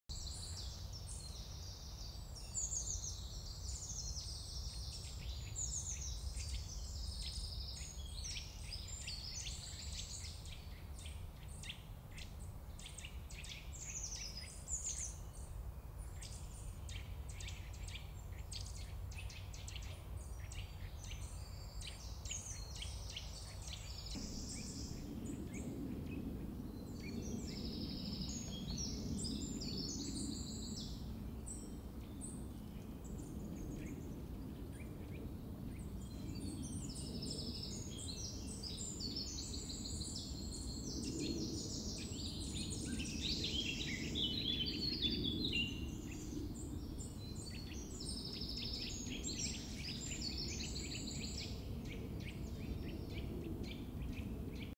Sterile leaves of fern 🔴
Description (more): A group of King ferns seen from a low position. From this same point of view is then a sheet to see from below. Composite clip.